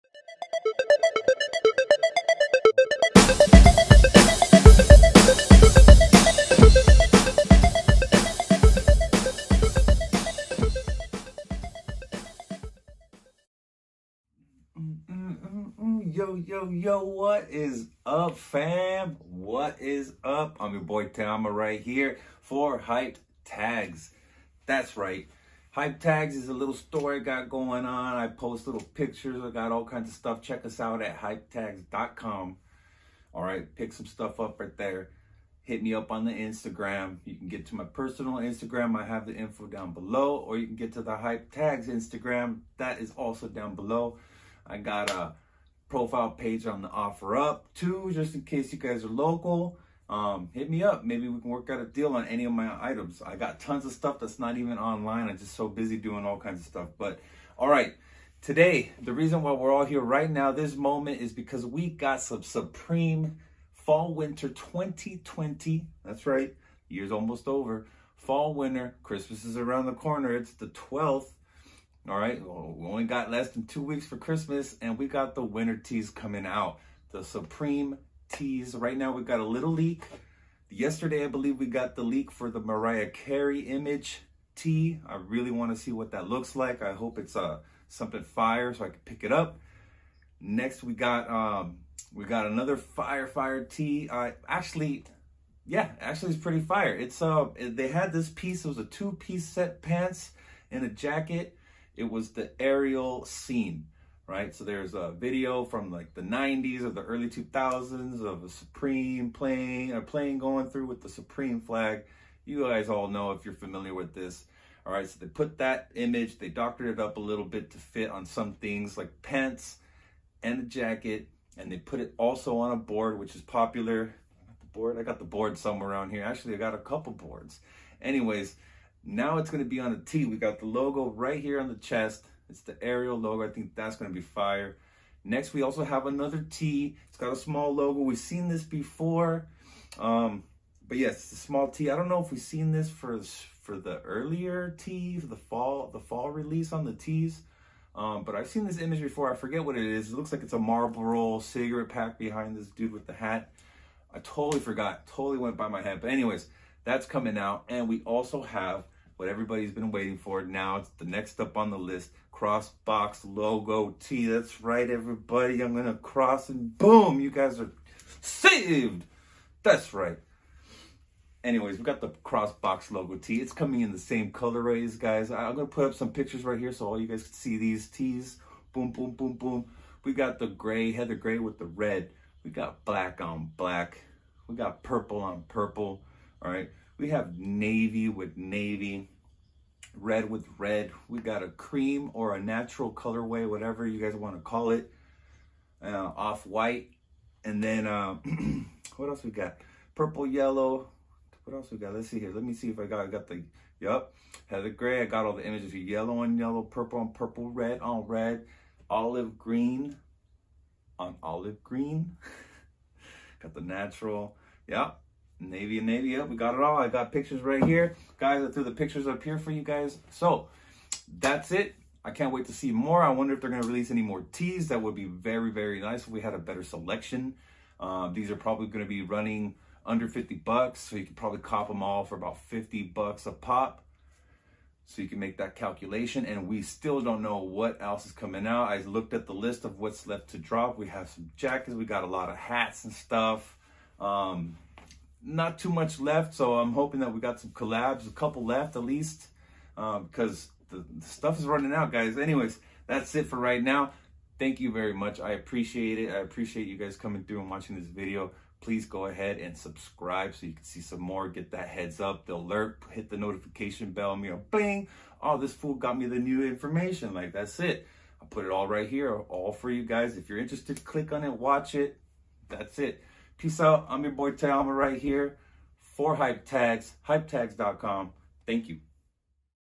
Supreme WEEK 17 winter tee’s
Update on the upcoming supreme FW20 winter tee’s. This is what’s out as of now Saturday evening before the week 17 drop.

Intro by T.AMA
Editing and Production by T.AMA